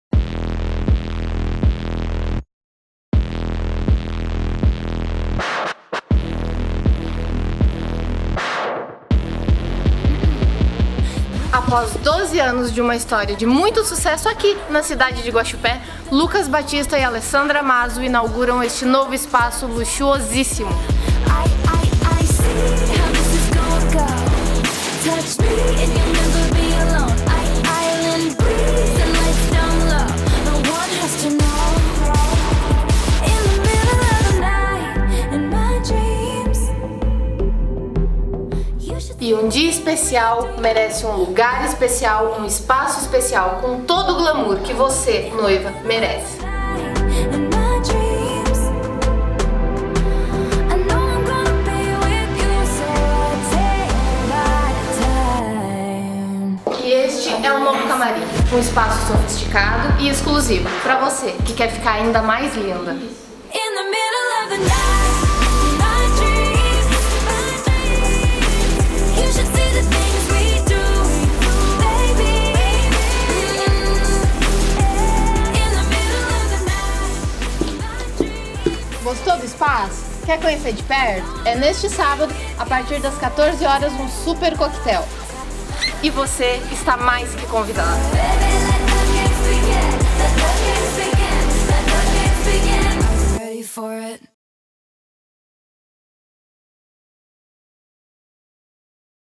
Conheça o novo espaço Lucas Batista Beauty Center e Le Mazzo Trajes Finos
Este é um convite, mais que especial, para você visitar o novo espaço localizado na Rua Francisco Vieira do Valle, 470, centro de Guaxupé (MG).

A partir de hoje (14/04). Início: 14:00

Você é o nosso convidado!

Produção: GMinas TV
Apresentação: Maiqueline Rosa
Direção/edição: João Paulo Angelini Marcos Vieira